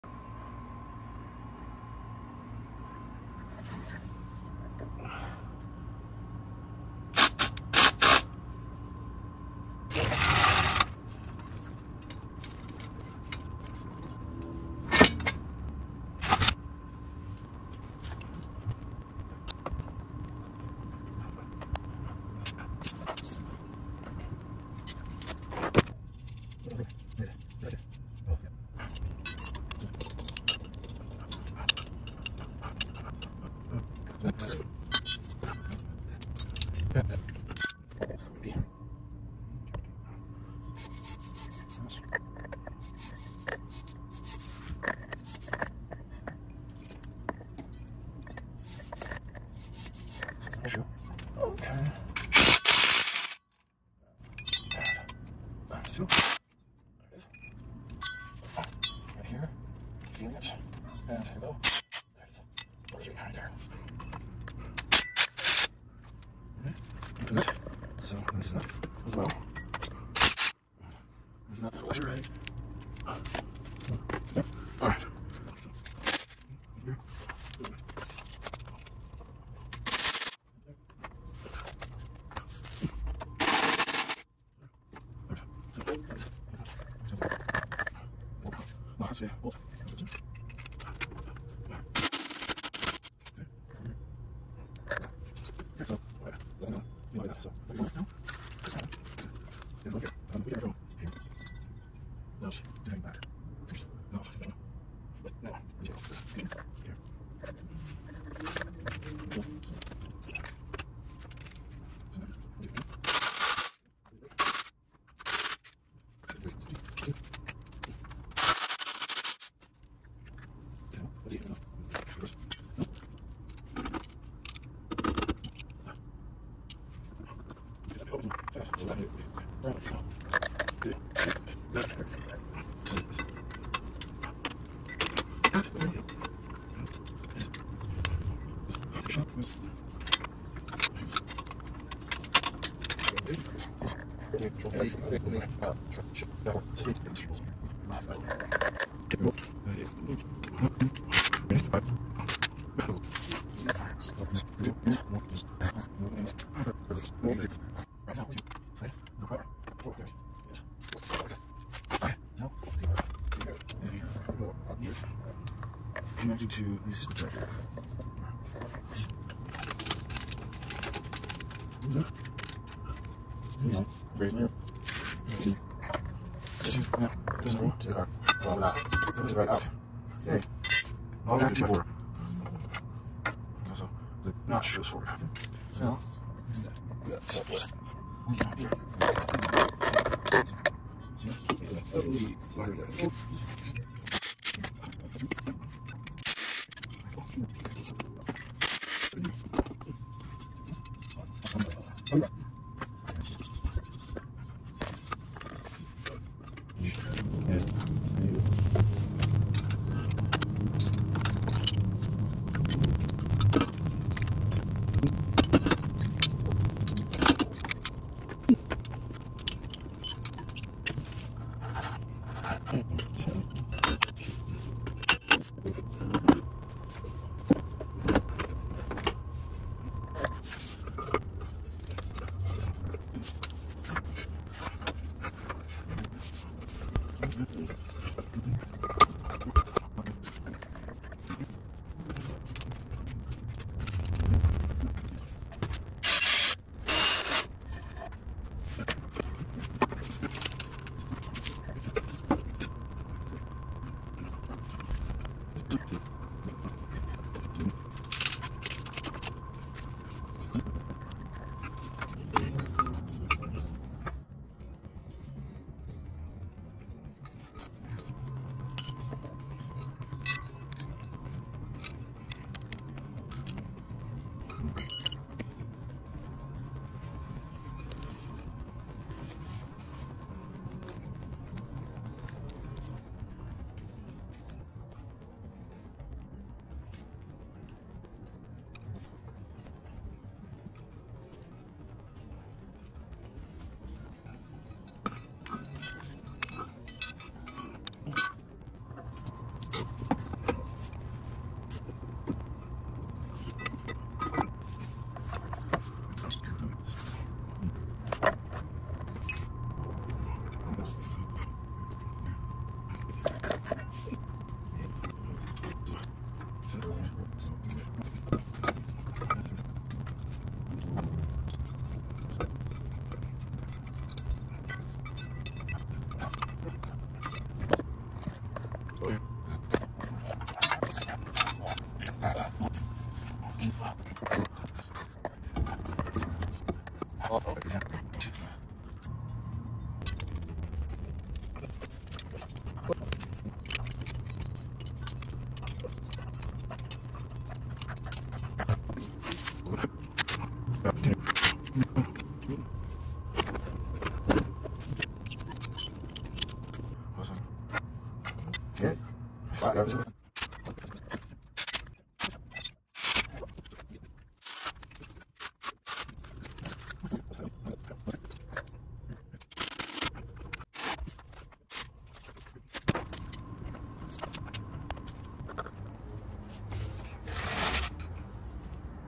1995 AWD Toyota Previa SADS Replace Timelapse
In this video, my hope of removing and installing the SADS with both couplers installed on the shaft is crushed, but I learn to deal with it.